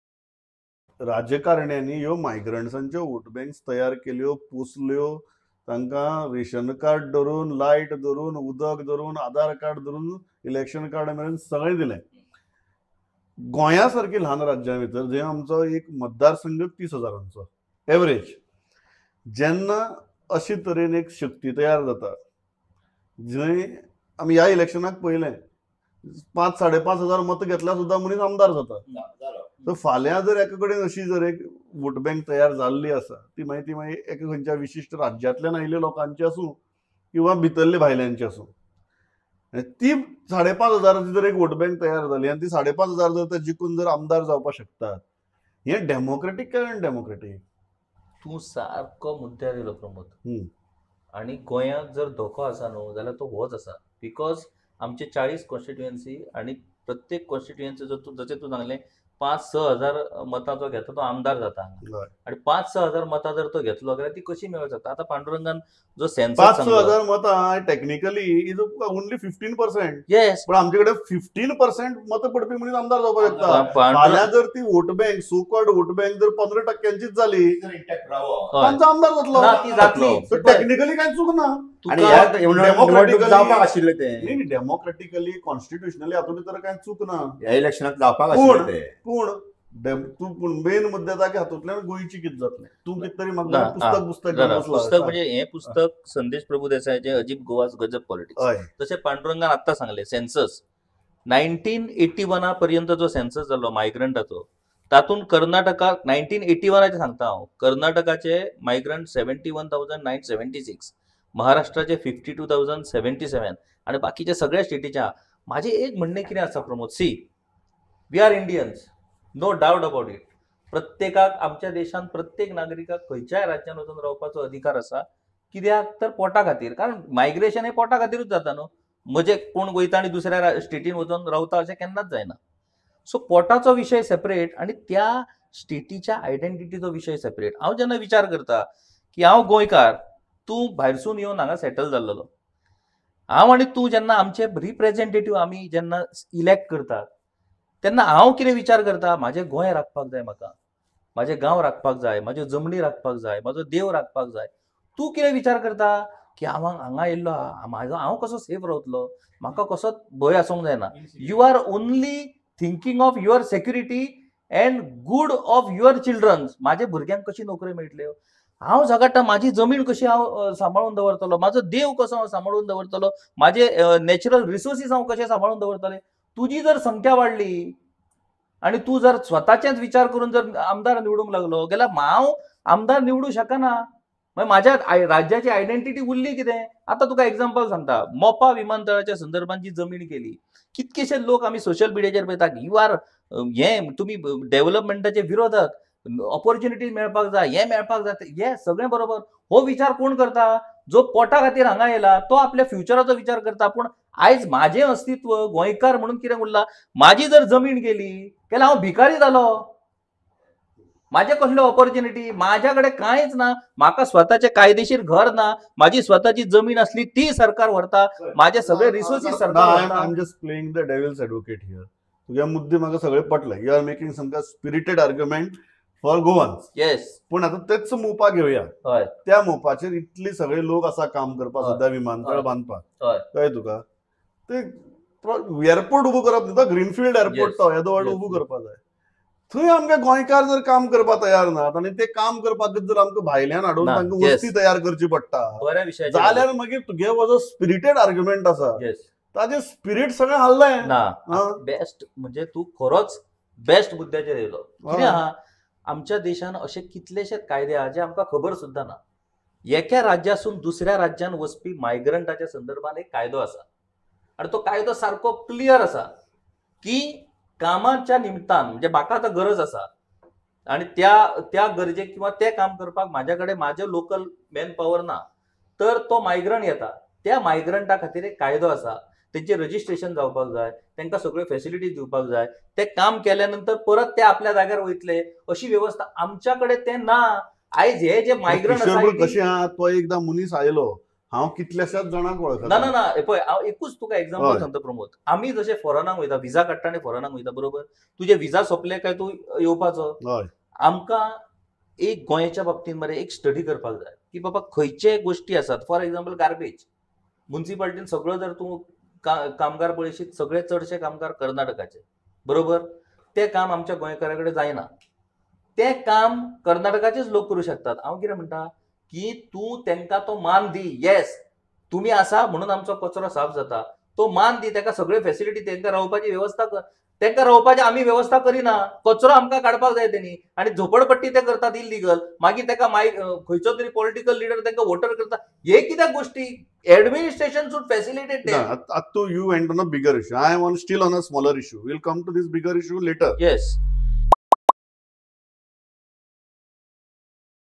Migrant Vote bank vs Goan voters | Round Table | Prudent
Watch the glimpses of Round Table conversation with our eminent panel on governments own statements about exposing itself.